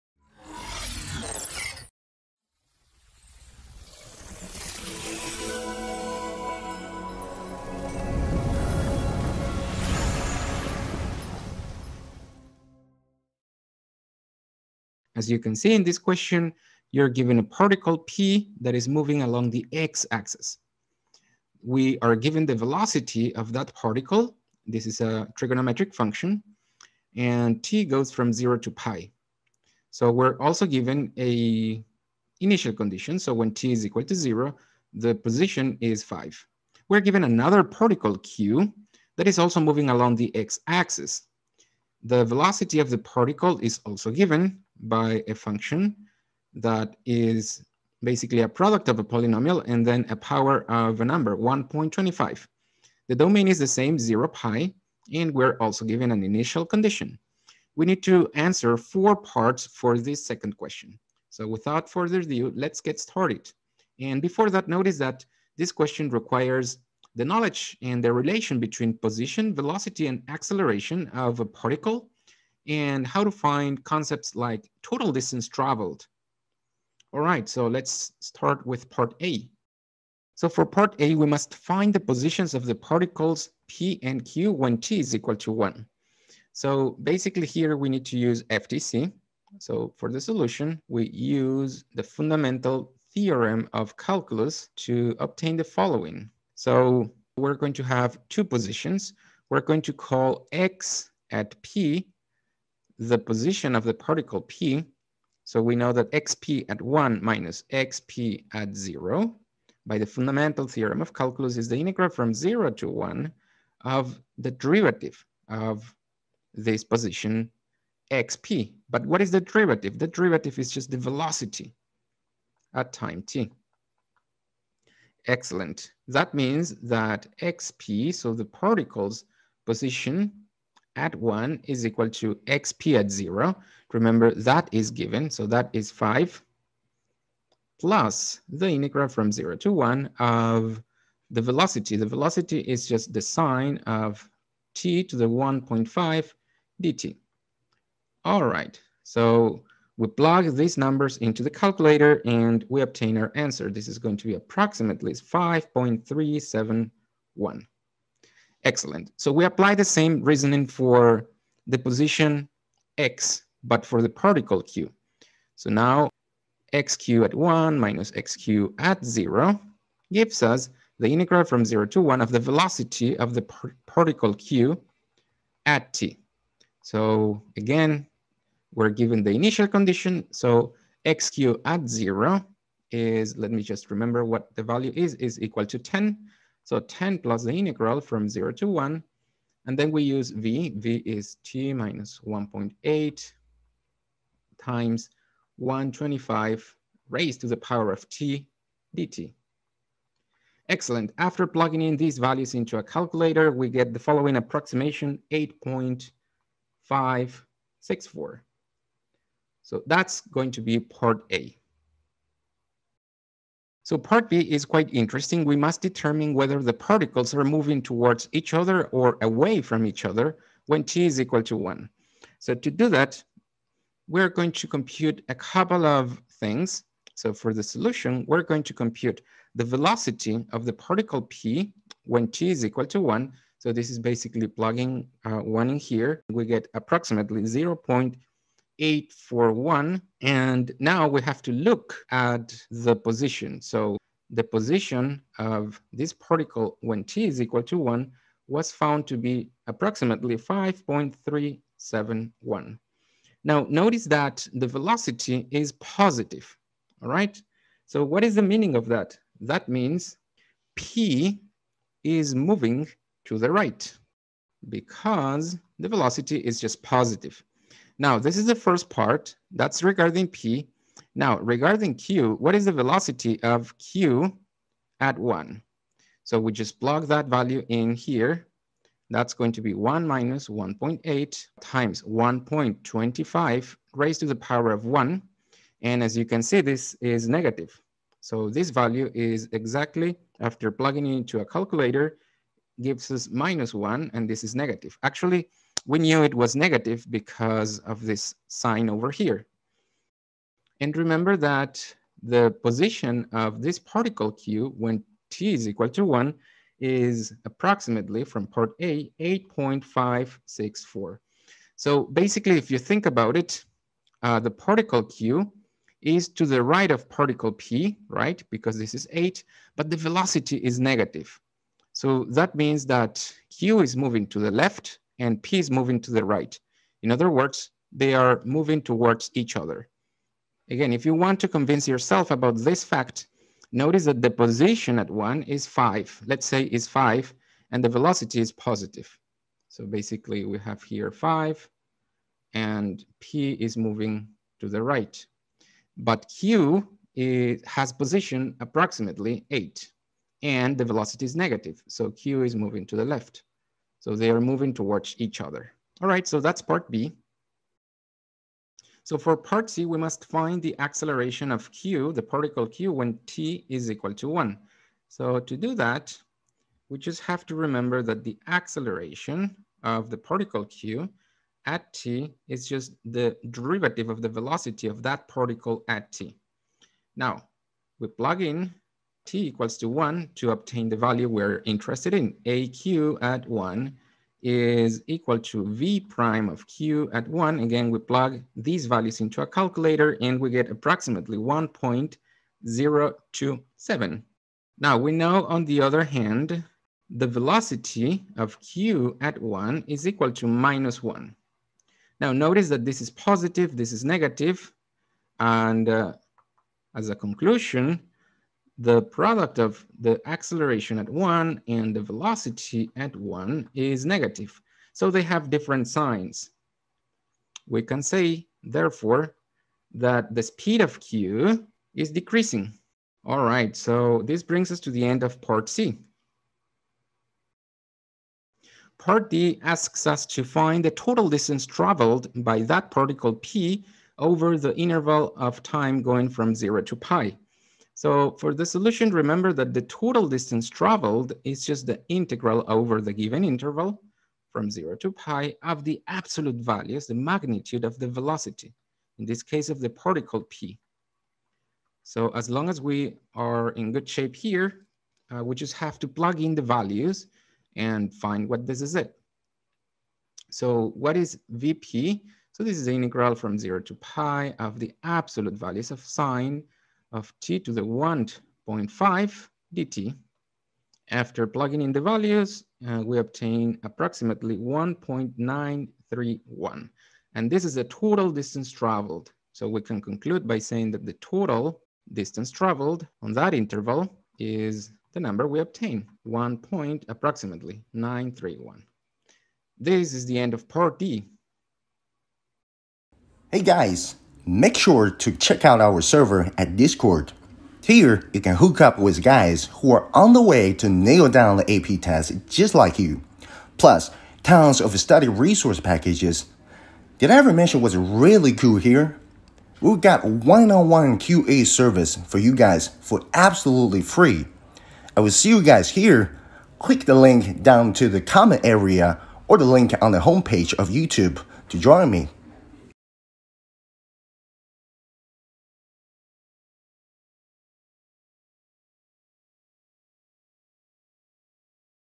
2021 AP Calculus AB FRQ #2 Full Video Solution
This video covers the FRQs from the May 2021 AP Calculus Exam.